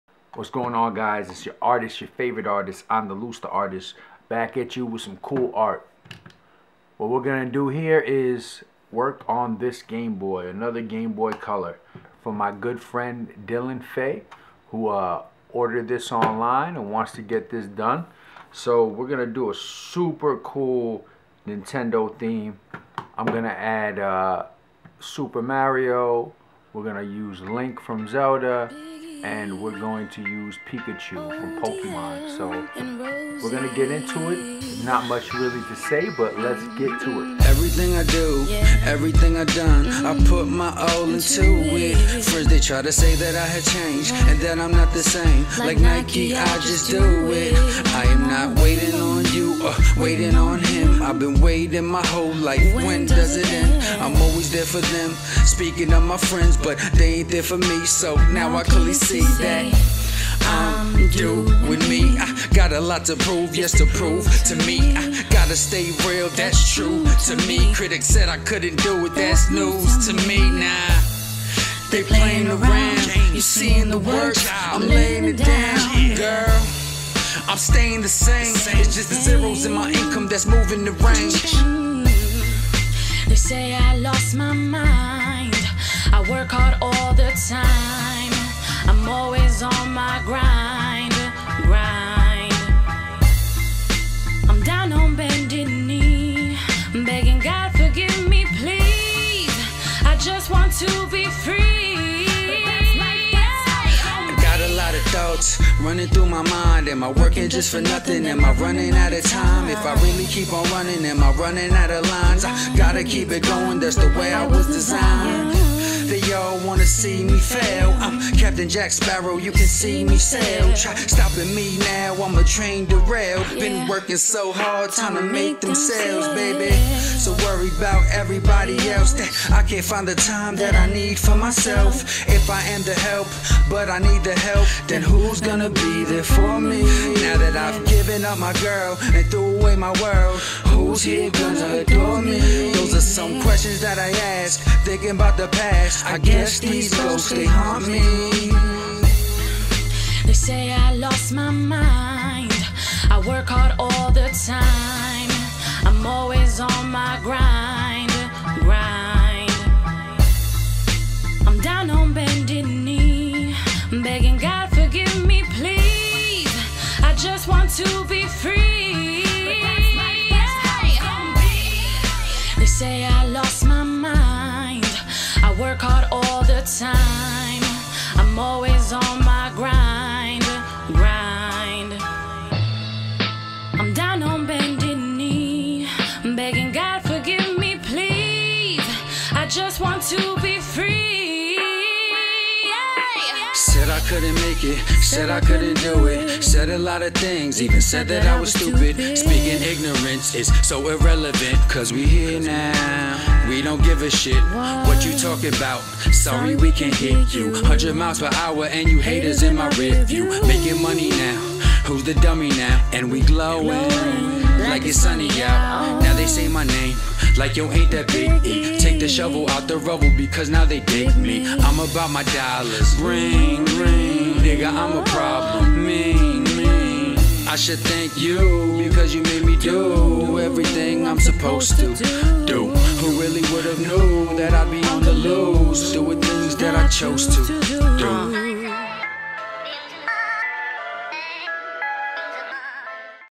Painted Nintendo Themed Gameboy Color (Super Mario, Link, Pikachu)
And another one! A client of mine wanted a Smash Bros. theme painted on a gameboy color so this is what I did. If you love Nintendo then you'll love this.

Follow Andaluz The Artist:
Facebook.com/AndaluzTheArtist

Song by: Andaluz The Artist feat. Rosie